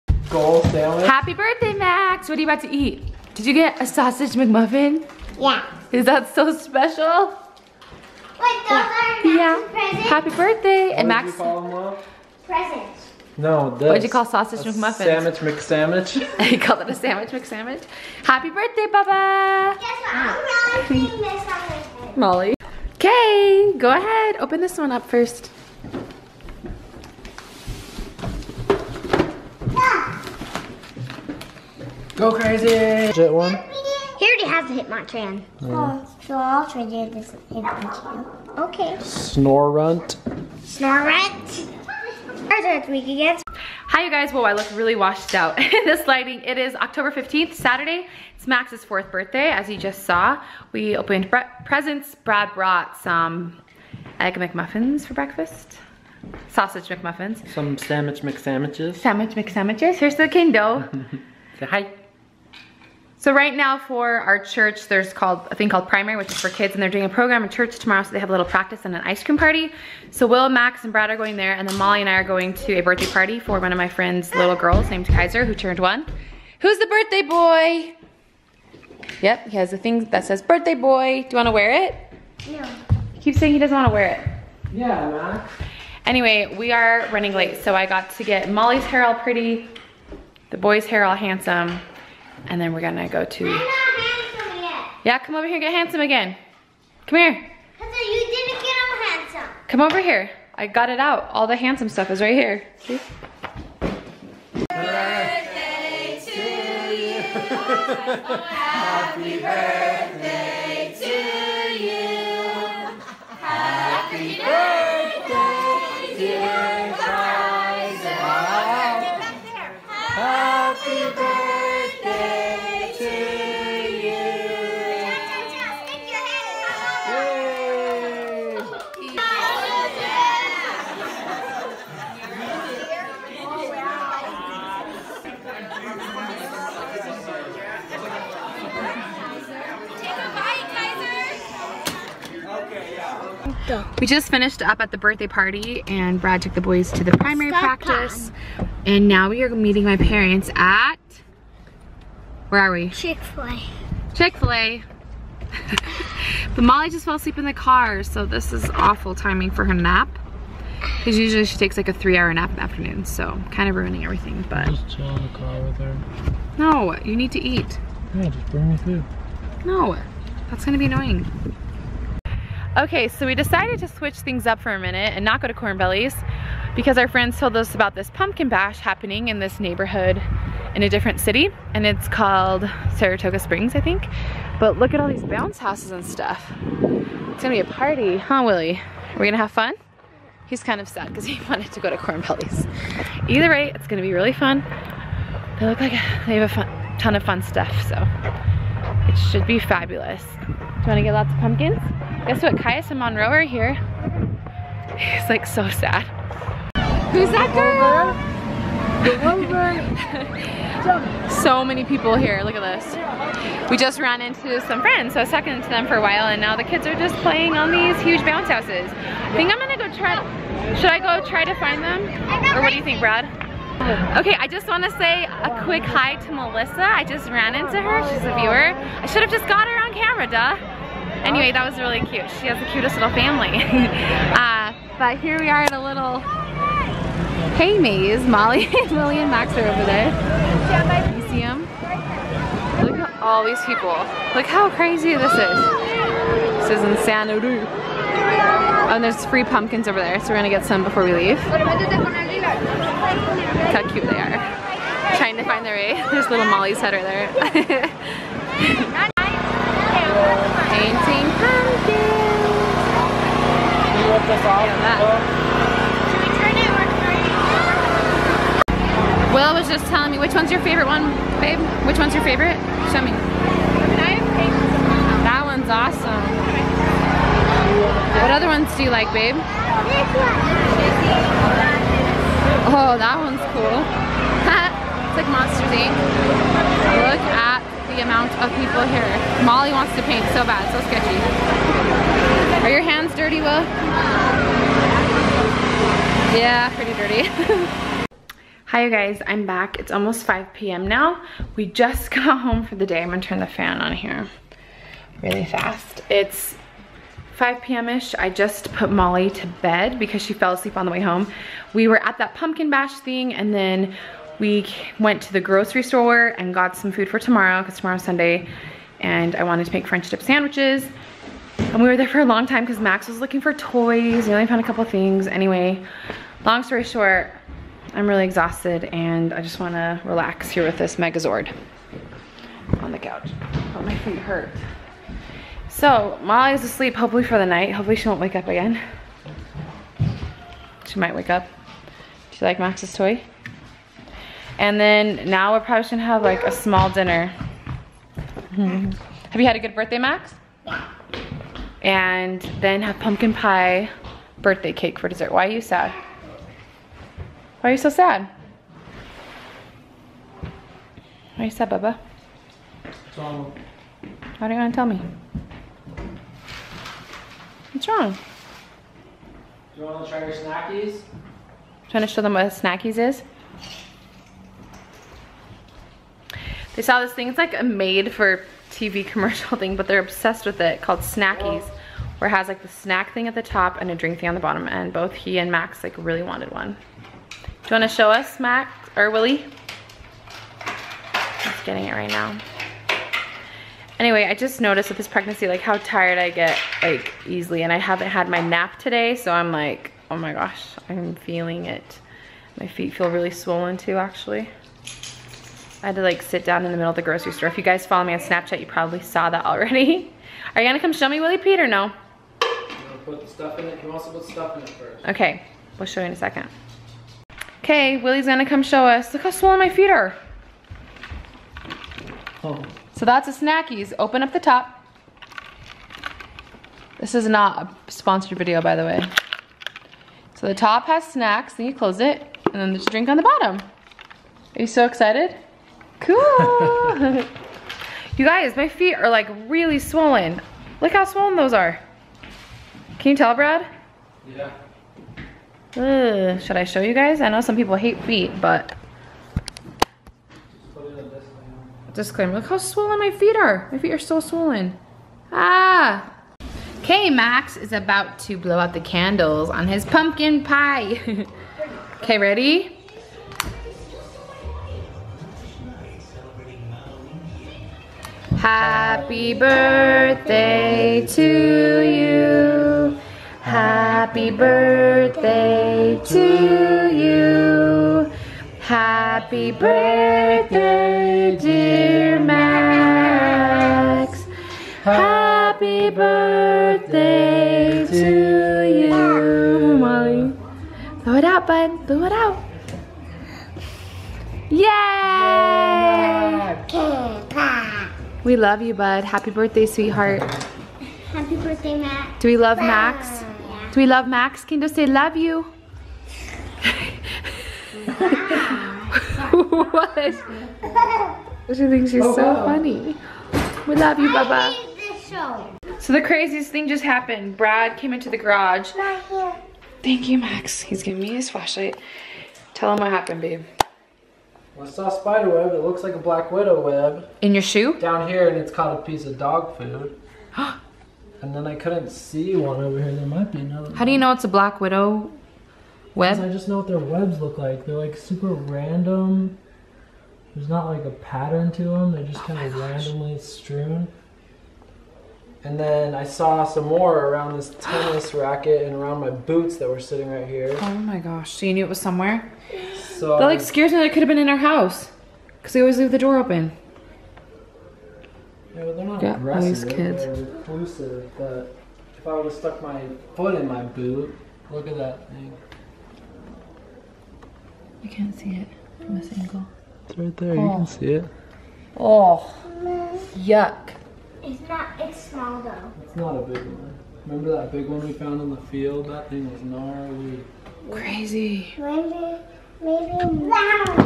MICHELLE PEARSON VLOG-MAX'S 4TH BIRTHDAY!
*On Saturday October 15th we celebrated Max's 4th Birthday! Follow us as we celebrated our spicy little tomato. I hope this video holds you over until we share our twins' genders tomorrow! Enjoy!